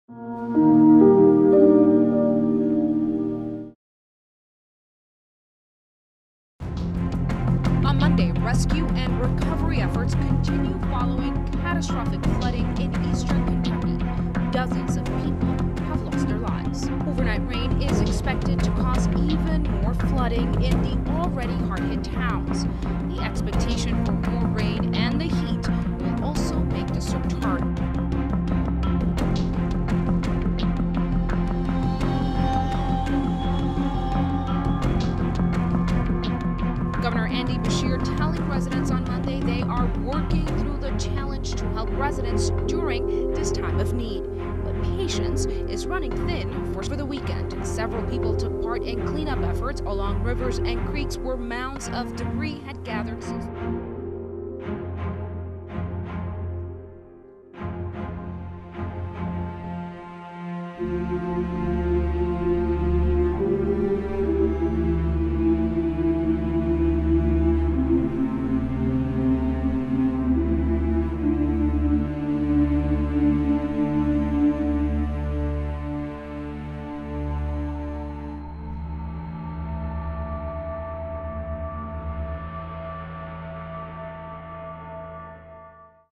35 Warga Tewas Saat Banjir Rendam Kota Kentucky AS
Korban banjir yang disebabkan hujan lebat di Kentucky Timur, Senin waktu setempat, bertambah menjadi 35 orang. Termasuk empat orang anak. Banjir ini adalah bencana nasional besar kedua yang melanda Kentucky dalam tujuh bulan terakhir.